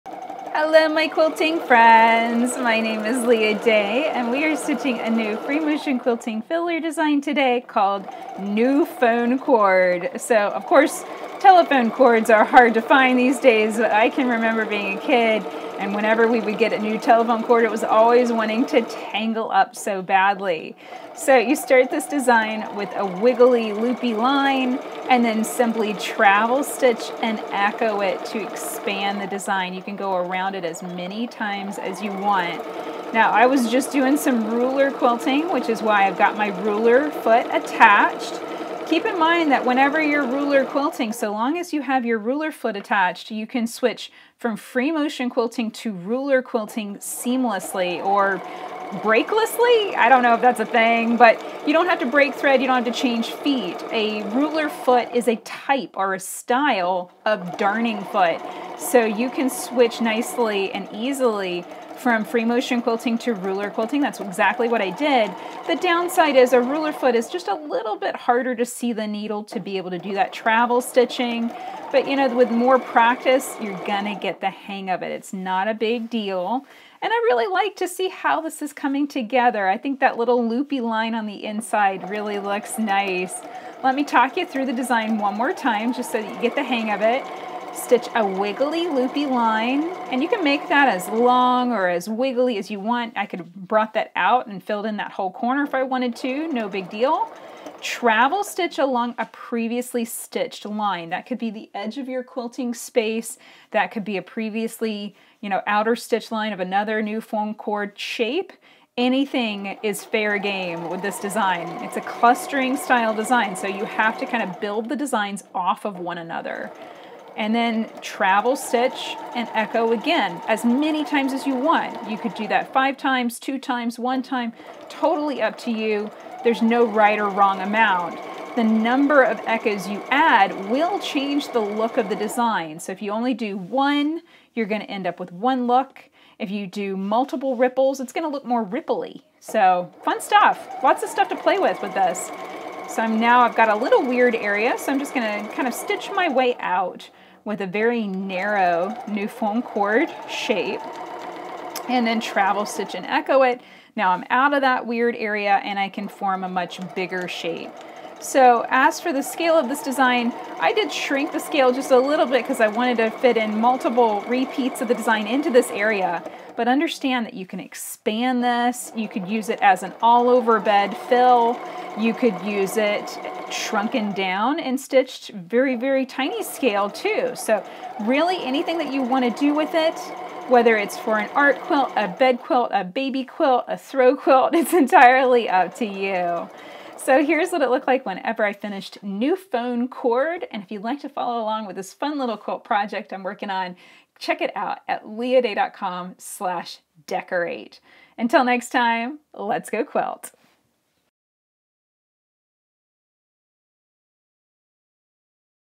Easy Free Motion Quilting - New Phone Cord Pattern for Beginners!
Here's an easy free motion quilting pattern I'm calling New Phone Cord - perfect for beginners! See how to machine quilt this pattern on a home sewing machine.

This video is part of the Decorative Stitch Quilting Mini Series. We're learning how to quilt with decorative stitches and how other types of machine quilting like free motion quilting and ruler quilting work together in the same project.

Deluxe Foot Set - this foot set includes walking feet, darning foot, AND a ruler foot!

Know which decorative stitches to use with the Decorative Stitch Swatcher Template

Jump ahead to your favorite part of this easy free motion quilting tutorial:
00:00 Machine quilting on a home sewing machine
00:20 Easy steps to quilting this pattern
00:33 Free motion quilting with a ruler foot
01:26 All the steps to quilting New Phone Cord
02:23 How to quilt a weird area without getting stuck in the design